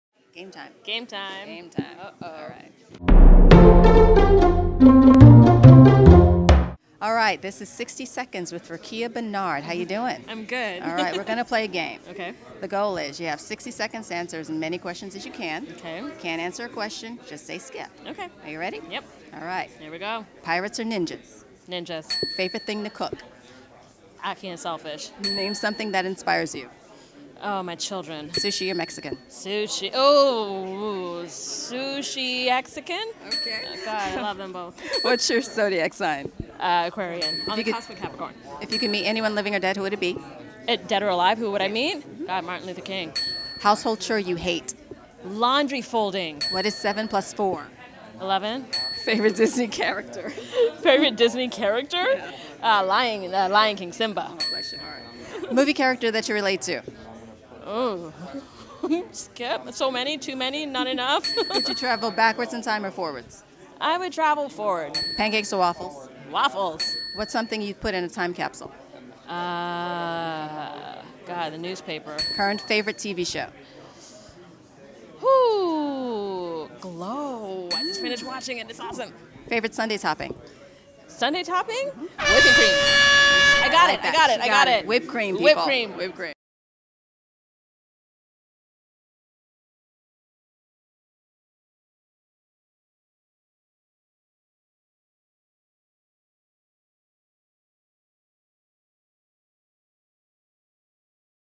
60 Seconds with Van Helsing's Rukiya Bernard
Welcome to 60 Seconds With... where you get to know your favorite actors/musicians and the points don't matter! Without further ado, please welcome Rukiya Bernard.